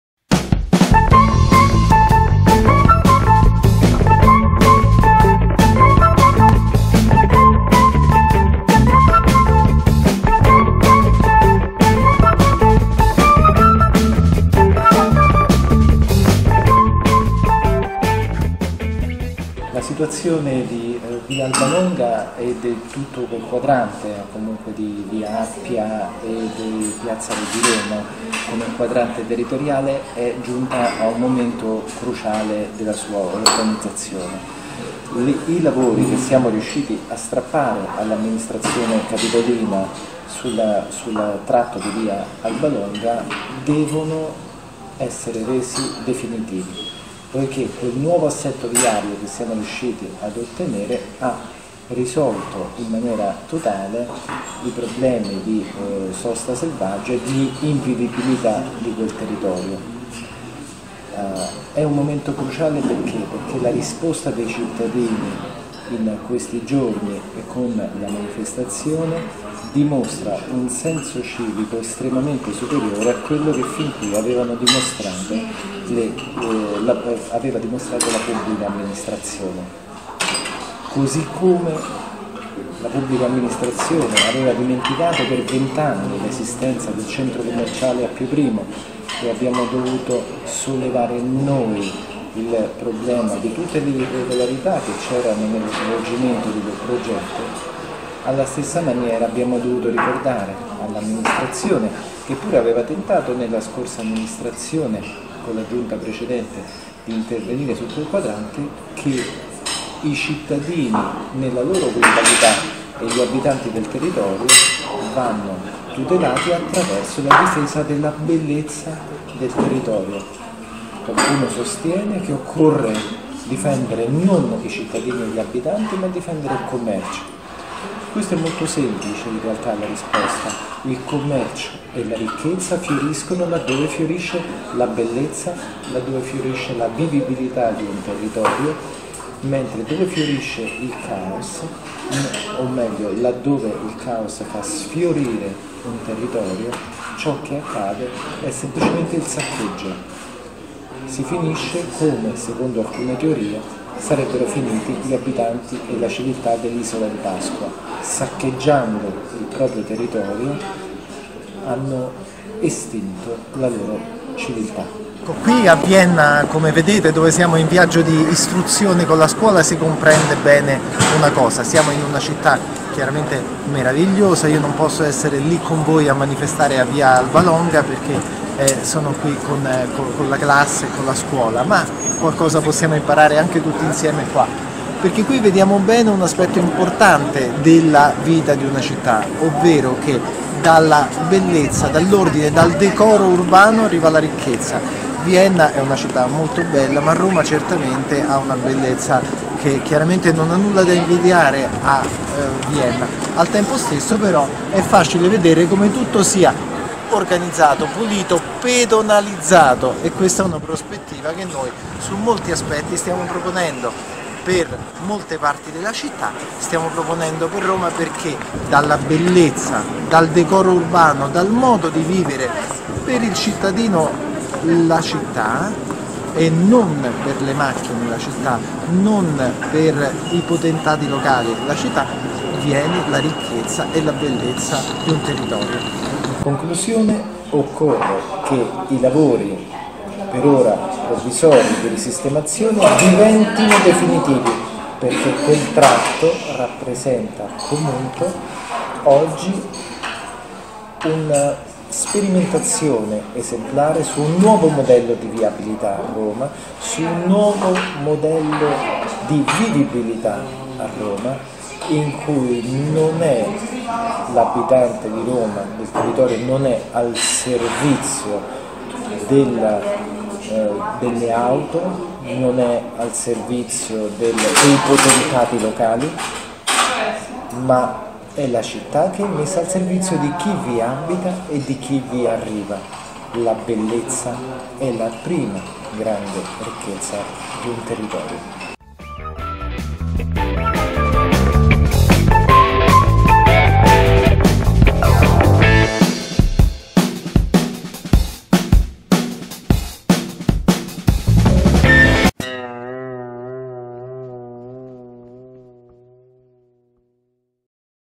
lascialadoppia - Davide Tutino, Consigliere del Municipio Roma VII per la Lista civica Marino
" lascialadoppia ": mobilitazione di cittadini e comitati in via Albalonga.

Intervista a  Davide Tutino, Consigliere del Municipio Roma VII per la Lista civica Marino.